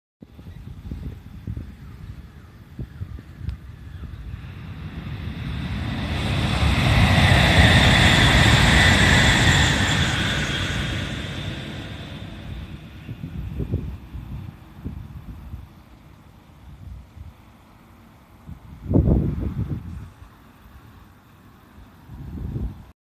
ドクターイエロー(試運転·下り)通過
ドクターイエローが通過してゆくところを撮影してみました

仕事の都合やら諸事情で撮影にいったり行かなかったりありましたがドクターイエローには会いに行ってみました(* ´ ▽ ` *)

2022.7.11